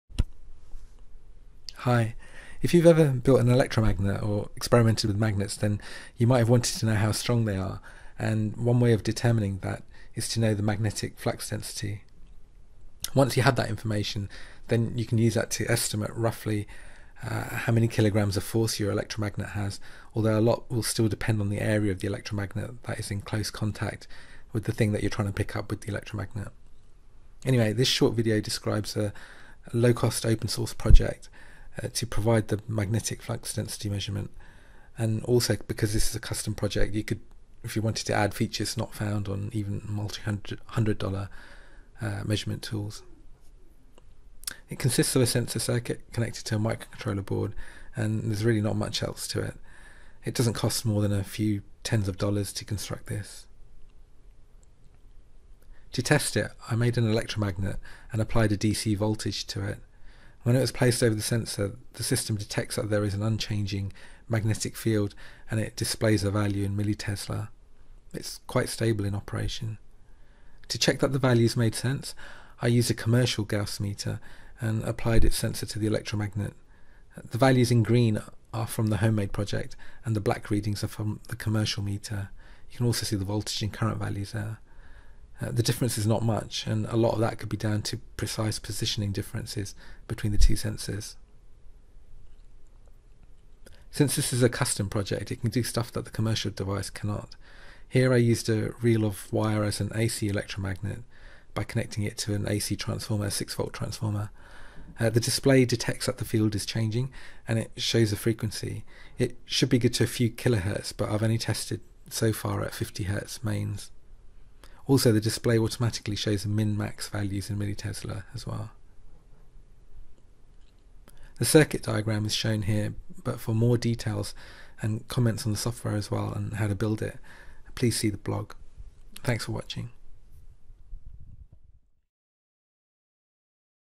Measuring Electromagnets - Building a Gaussmeter
A quick 2-minute explanation of an open hardware/open source Gaussmeter, for measuring magnetic flux density (in milliTesla, or in Gauss). For more detail and comments, please see the written blog post at element14,